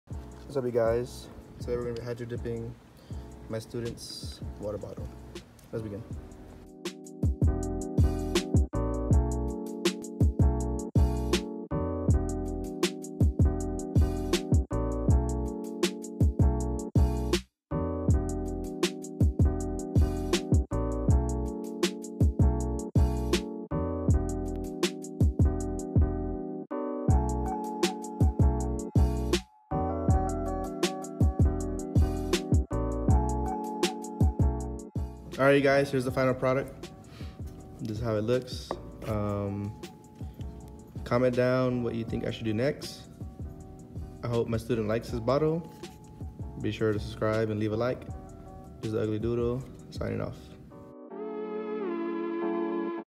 Hydro Dipping My Student’s Water Bottle!
Hello everyone! So one of my students asked me if I could hydrodip his water bottle and of course I said yes! 👍🏻